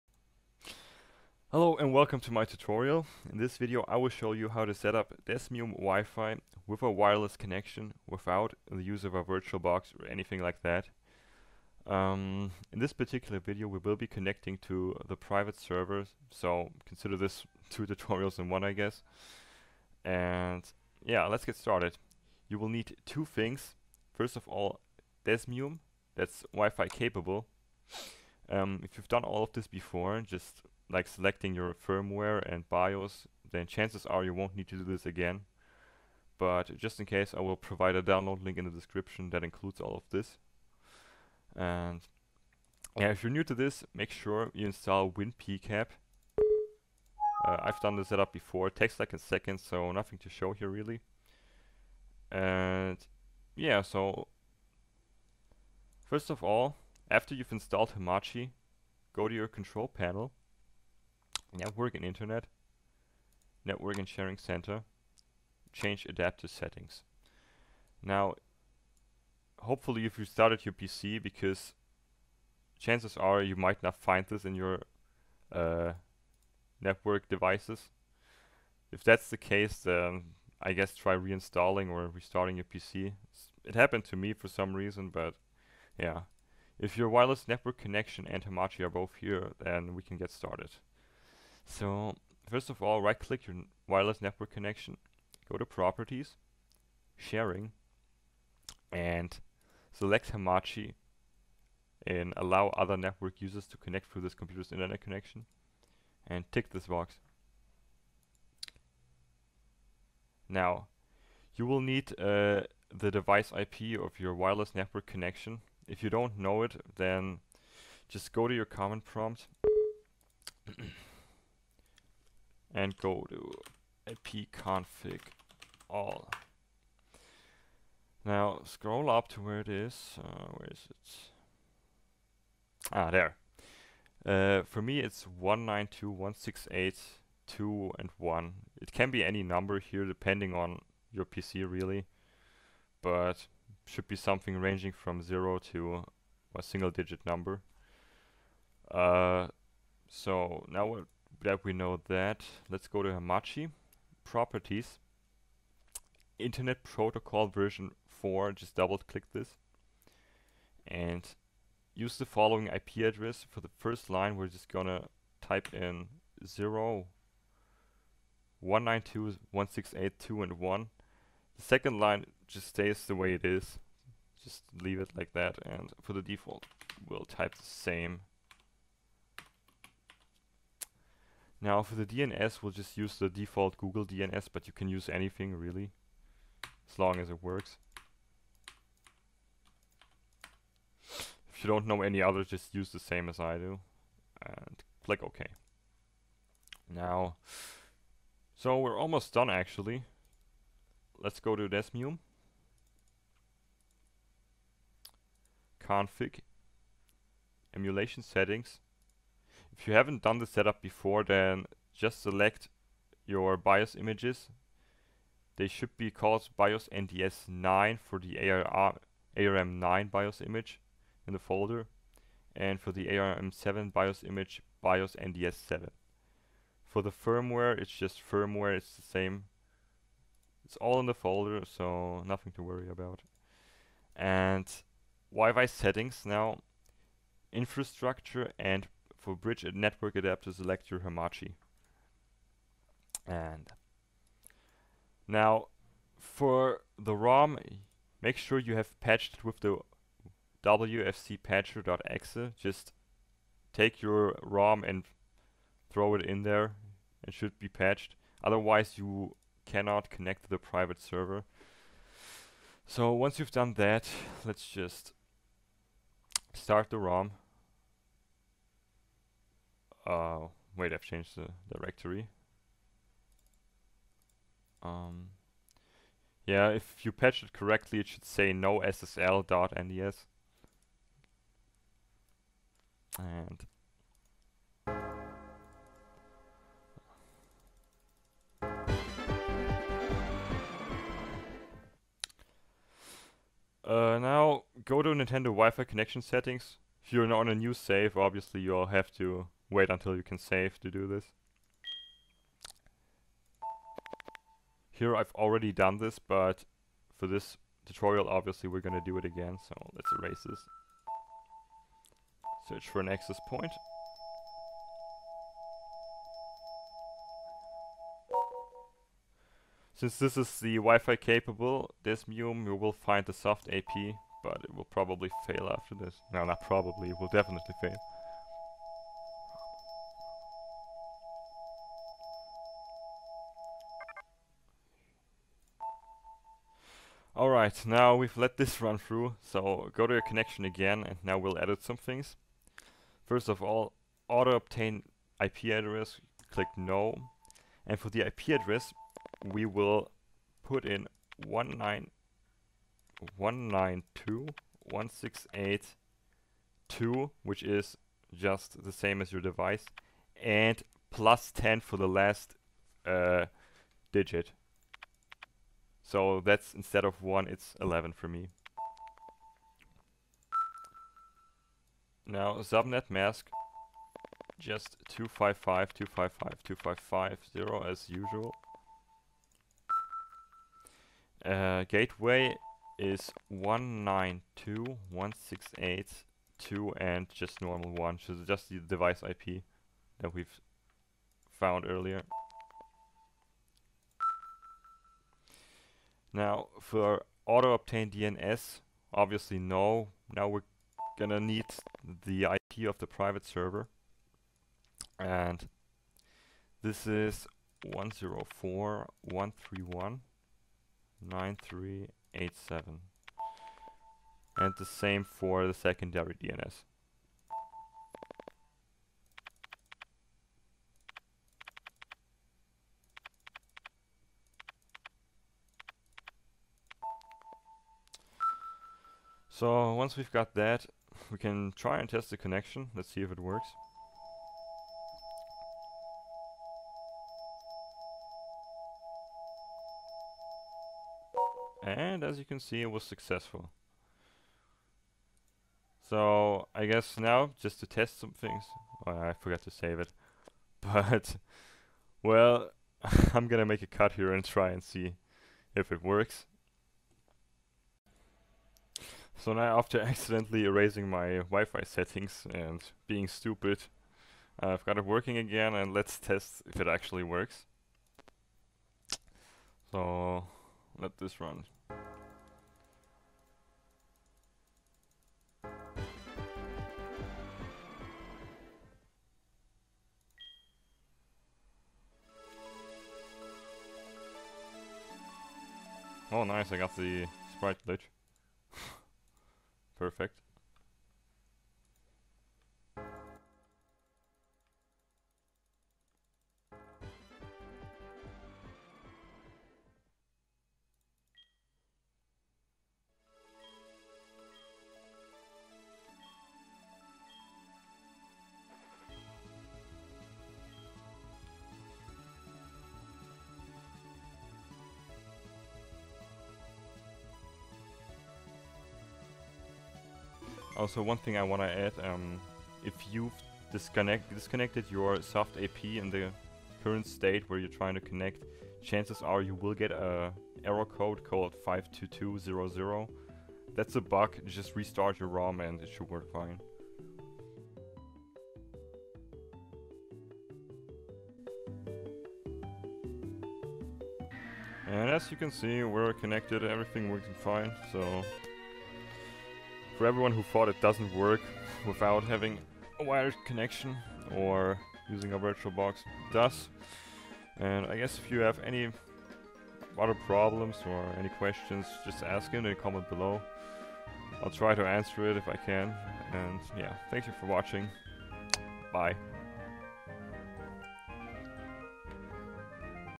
DeSmuME WiFi on a WLAN connection (NO VIRTUAL BOX)
How to setup DeSmuME WiFi using Hamachi